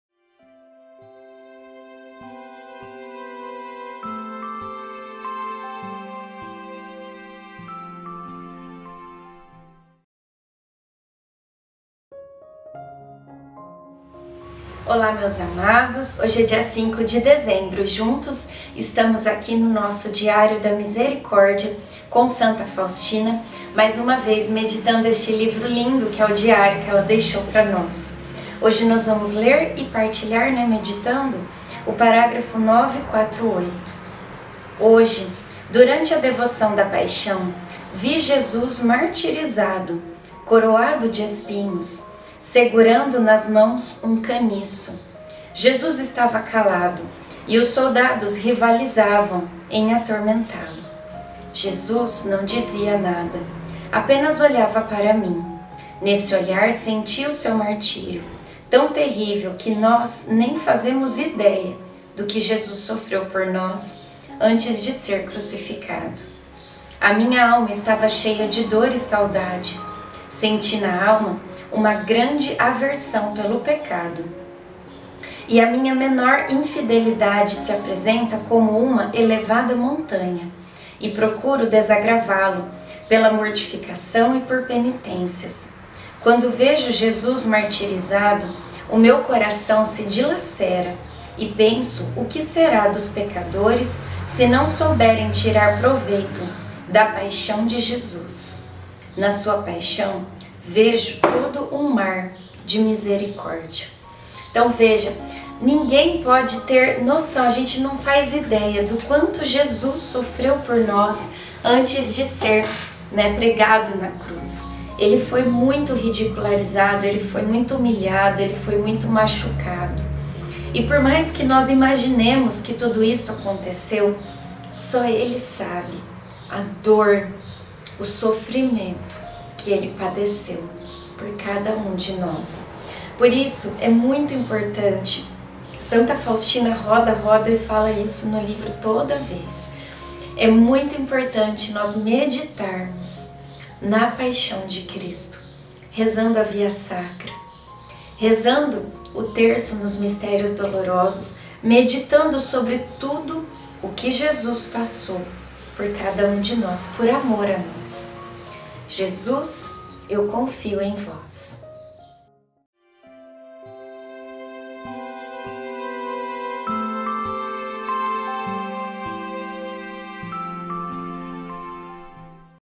DIÁRIO DA MISERICÓRDIA COM SANTA FAUSTINA 05/12/22
94813.02.[1937]. Hoje, durante a devoção da Paixão
[390]
, vi Jesus
martirizado, coroado de espinhos, nas mãos segurava um caniço.

Jesus permanecia calado e os soldados rivalizavam em atormentá-
Lo. Jesus não dizia nada, apenas olhava para mim. Nesse olhar

senti o Seu martírio, tão terrível que nós nem fazemos ideia, do que
Jesus sofreu por nós antes de ser crucificado. A minha alma estava
cheia de dor e saudade. Senti na alma uma grande aversão pelo
pecado e a minha menor infidelidade se apresenta como uma
elevada montanha, e procuro desagravá-Lo pela mortificação e por
penitências. Quando vejo Jesus martirizado, o meu coração se
dilacera e penso o que será dos pecadores se não souberem tirar
proveito da Paixão de Jesus. Na Sua Paixão vejo todo um mar de
misericórdia.

Deus abençoe!